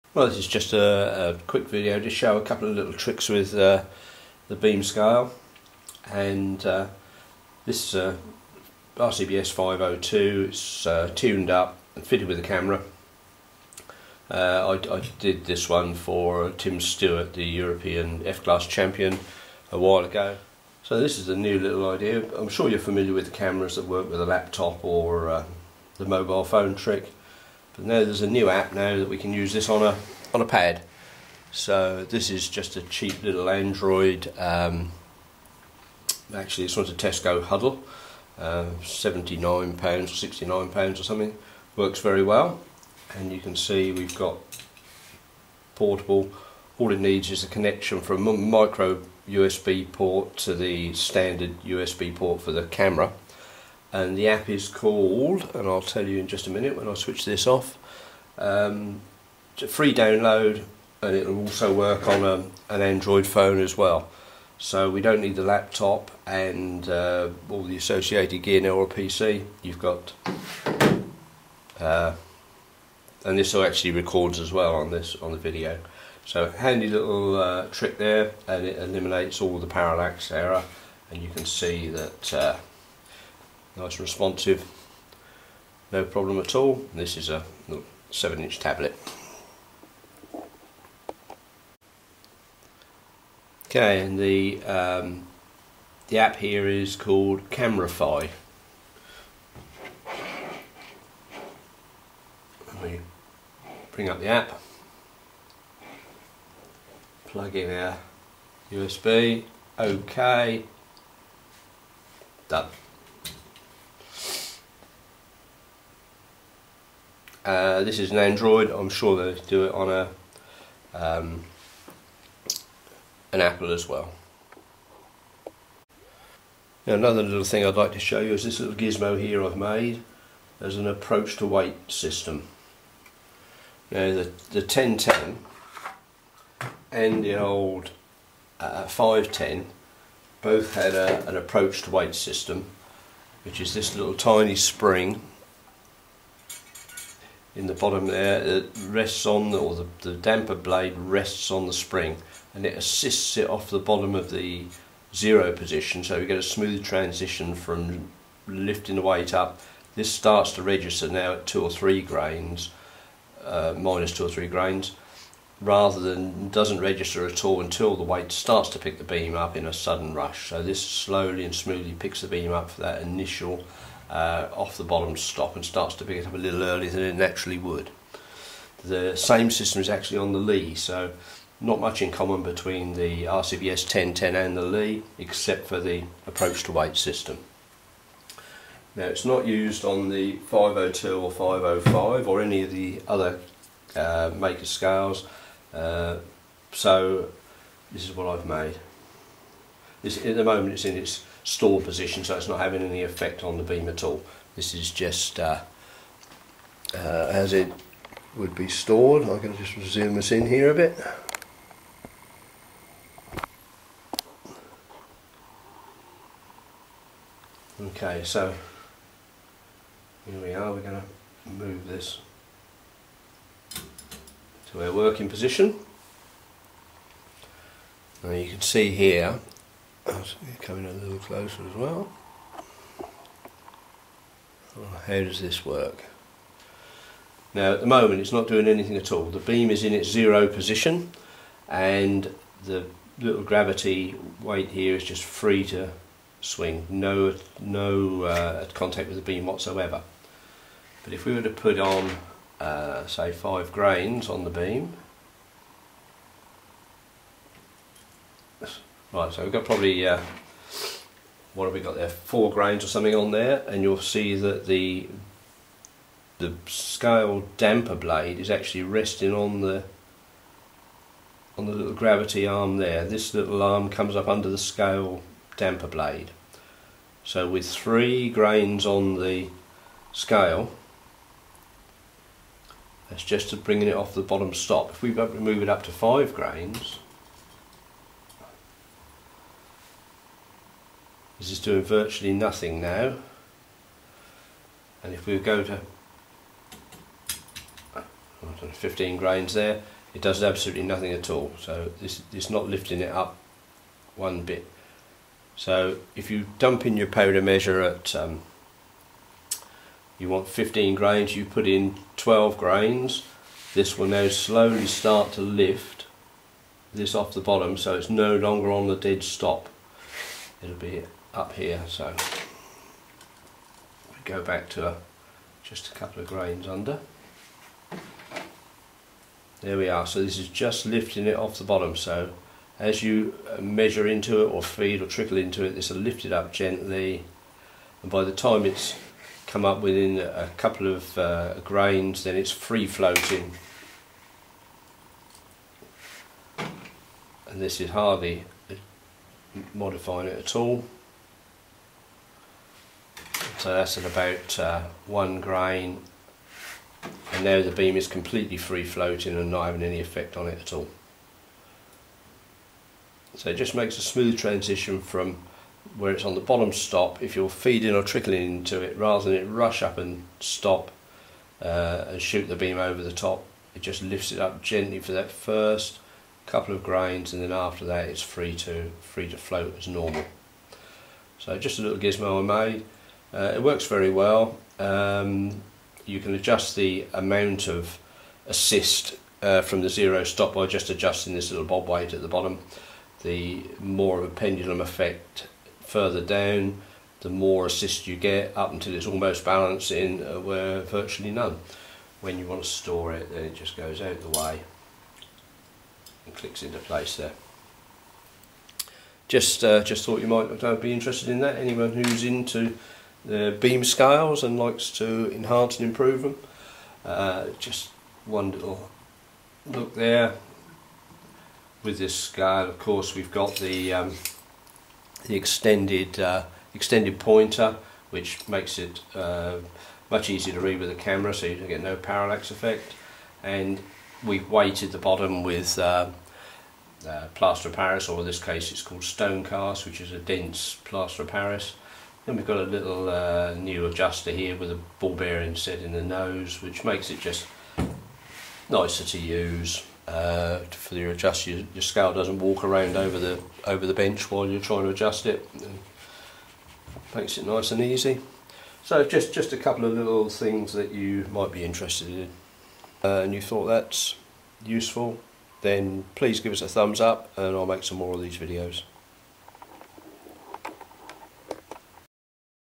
Beam scale enhancements
Just a couple of ideas that may help you get the best possible performance out of your beam scales.

Check my other scale videos if you are interested in beam scales.